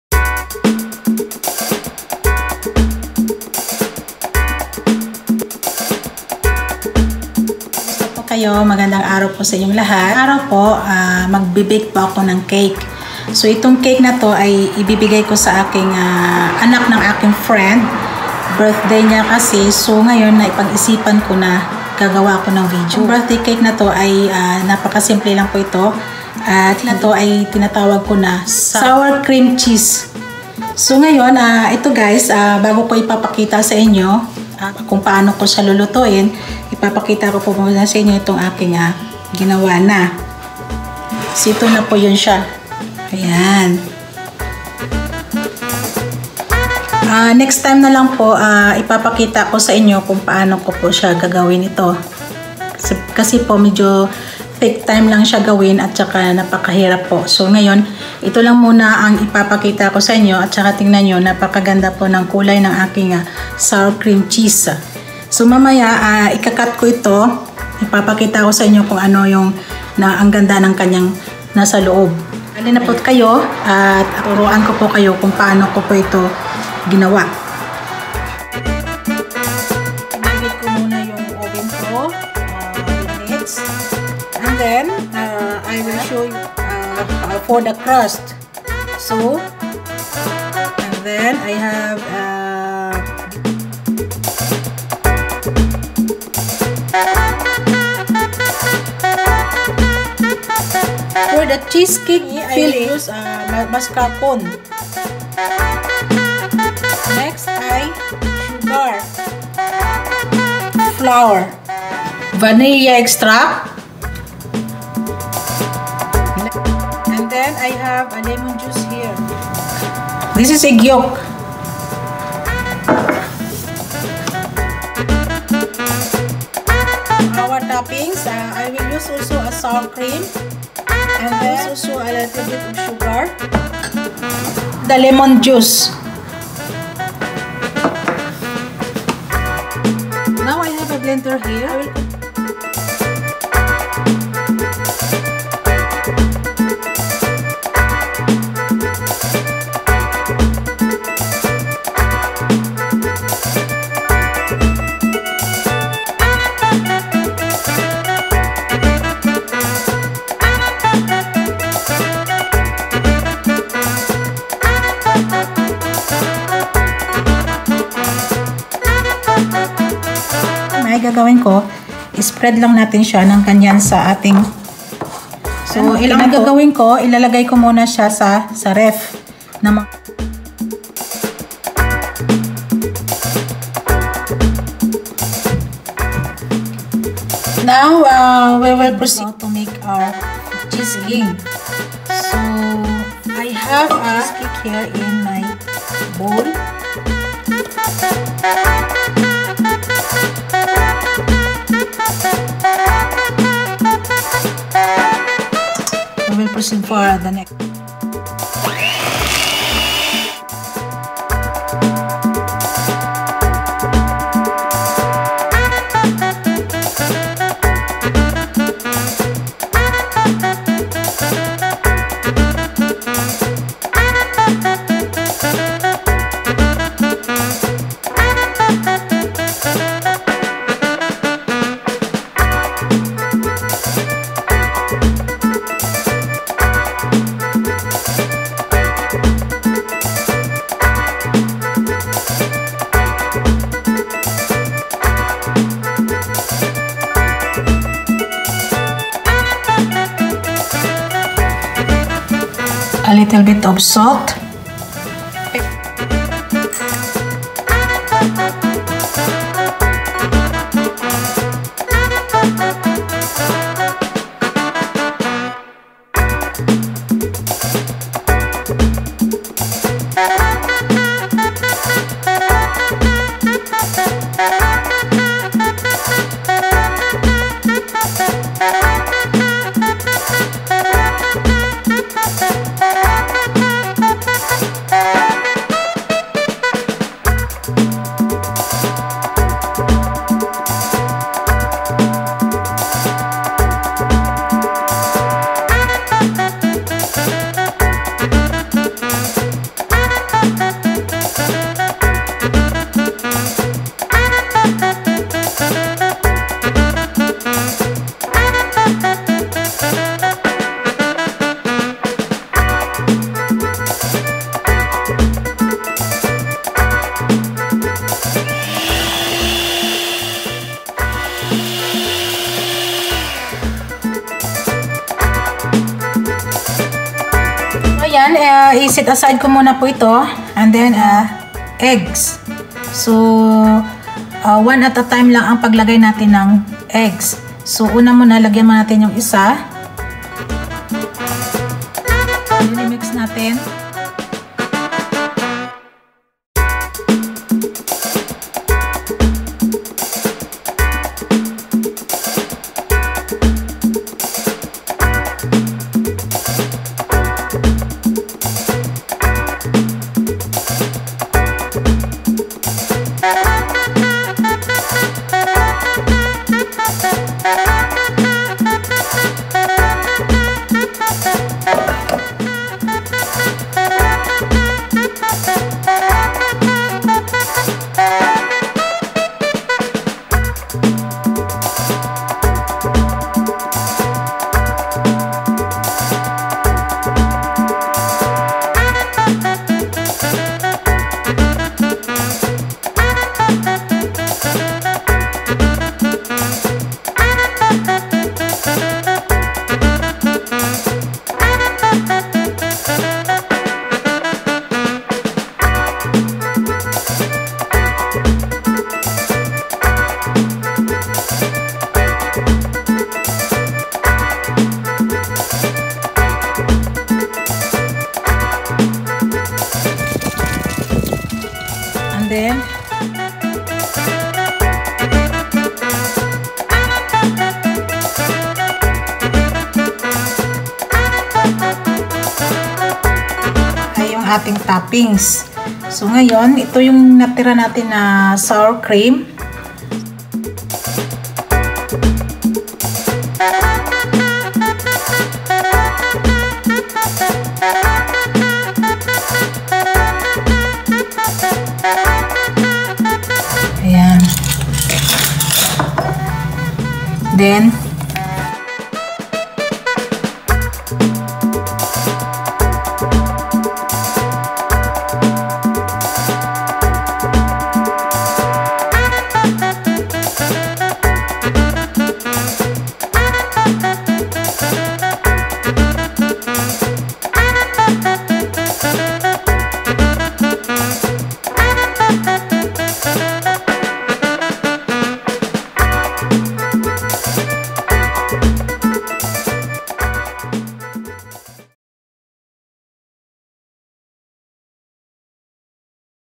Delicious Soured Cream Cheesecakes | MAGGIE IMBERT
Today I'll show you step by step how to  make The Soured Cream Cheesecake.

SOURED CHEESECAKE
 Ingredients:
 For the Crust
   150 g digestive biscuits or graham
    1 tbsp sugar
     80g butter

FOR THE CHEESECAKE FILLING
   900g mascarpone or Philadelphia cheese
   250g caster sugar
   1 1/2 tsp vanilla extract
    2 tsp lemon zest
    1 1/2 tsp lemon juice
    3 large eggs
    1 egg yolk
     290ml soured cream

FOR THE CHEESECAKE TOPPINGS
     150ml soured cream
     1 tbsp caste sugar
      2 tsp lemon juice

Happy baking!!!